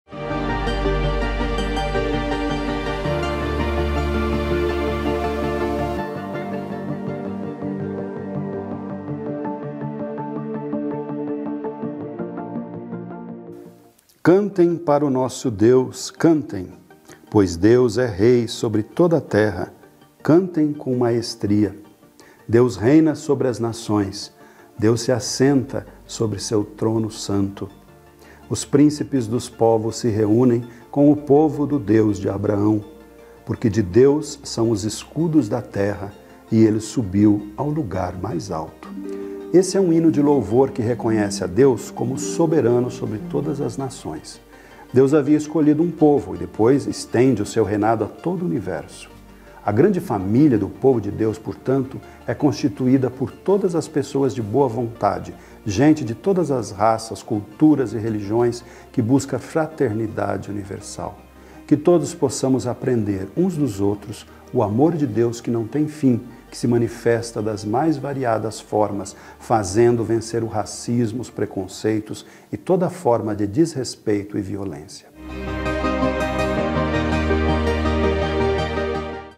Rezando com os Salmos「23.04.2023」 Pe. Paulo Bazaglia, ssp
“Cantem para o nosso Deus!”
SALMO 47,6-10

Este é um hino de louvor que reconhece a Deus como o soberano sobre todas as nações. Deus havia escolhido um povo, e depois estende seu reinado a todo o universo.  A grande família do povo de Deus, portanto, é constituída por todas as pessoas de boa vontade, gente de todas as raças, culturas e religiões que busca fraternidade universal. Que todos possamos aprender, uns dos outros, o amor de Deus que não tem fim, que se manifesta das mais variadas, fazendo vencer o racismo, os preconceitos e toda forma de desrespeito e violência.